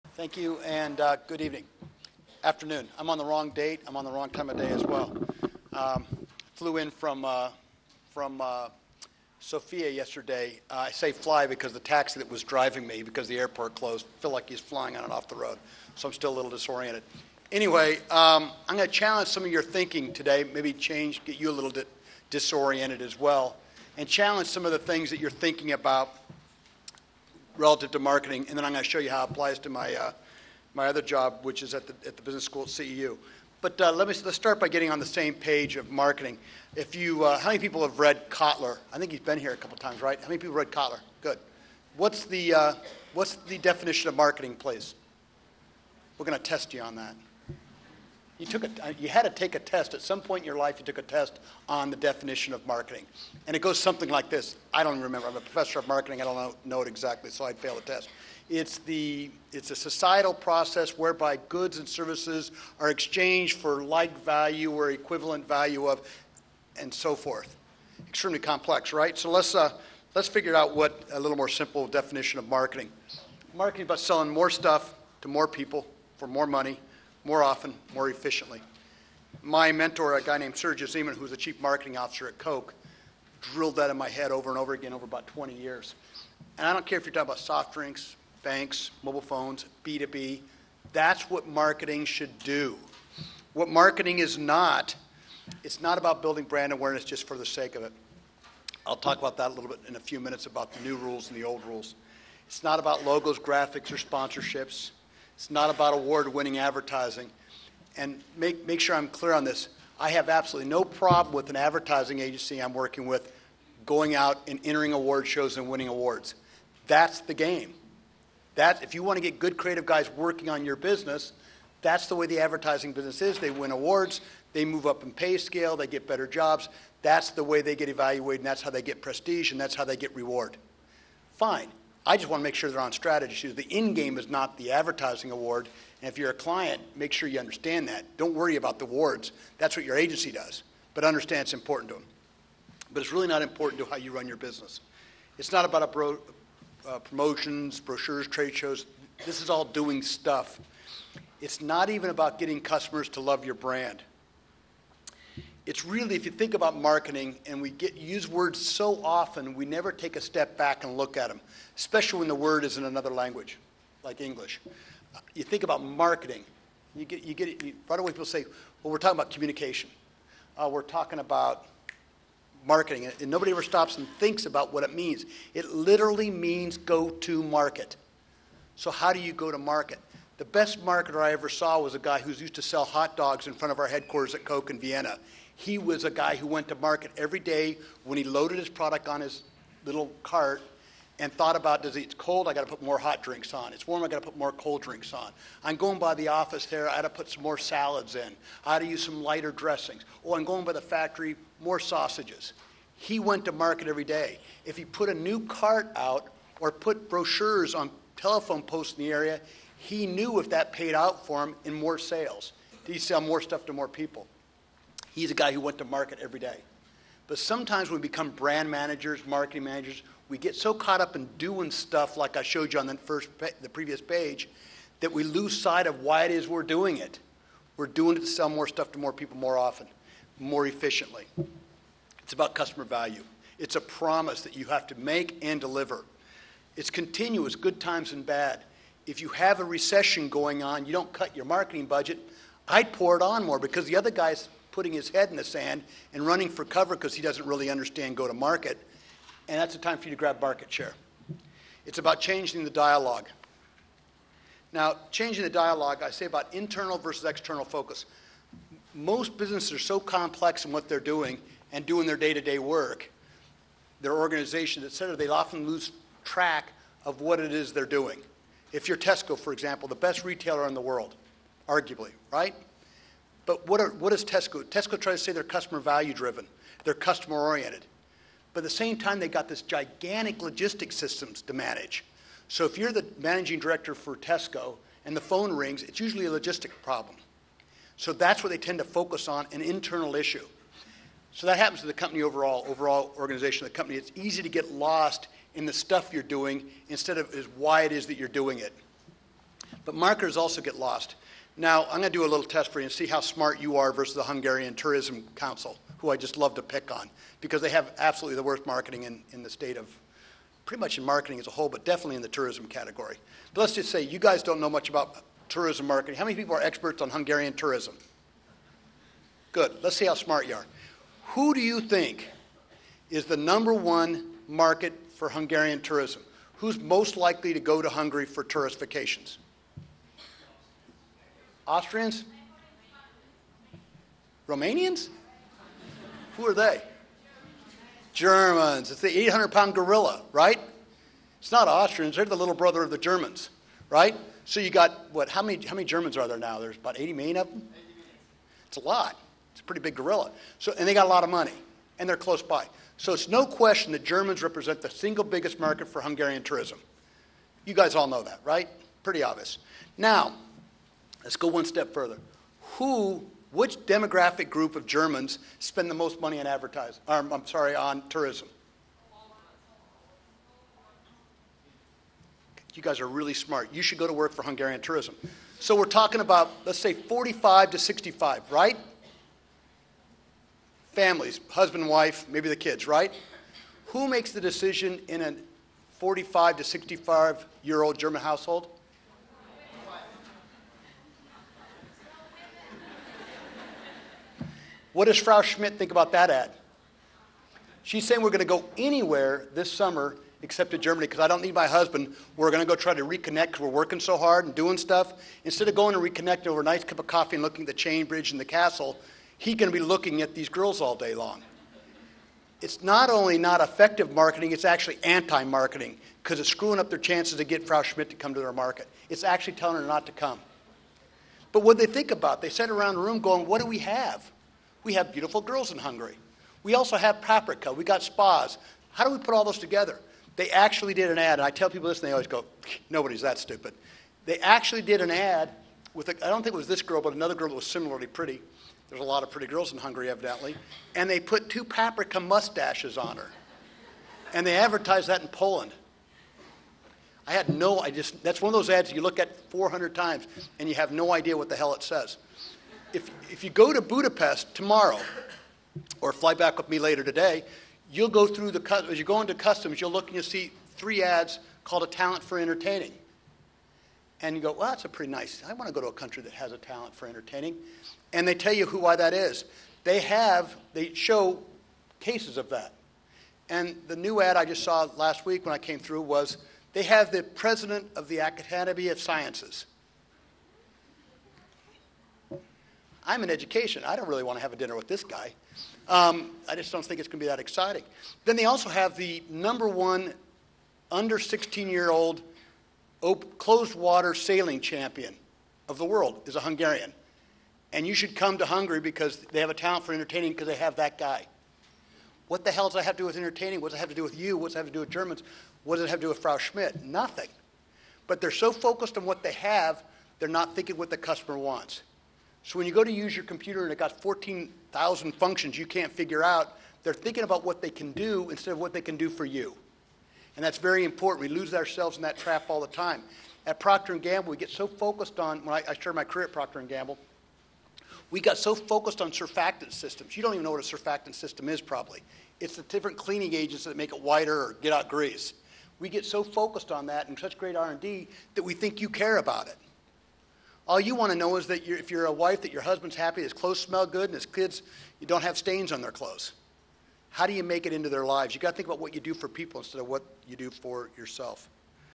Paul Garrison at BizDays
Former Managing Director of Coca-Cola Hungary and prior to that the Marketing Director for Coca-Cola CEE Europe, Paul is founder of Garrison Group and Dean at CEU Business School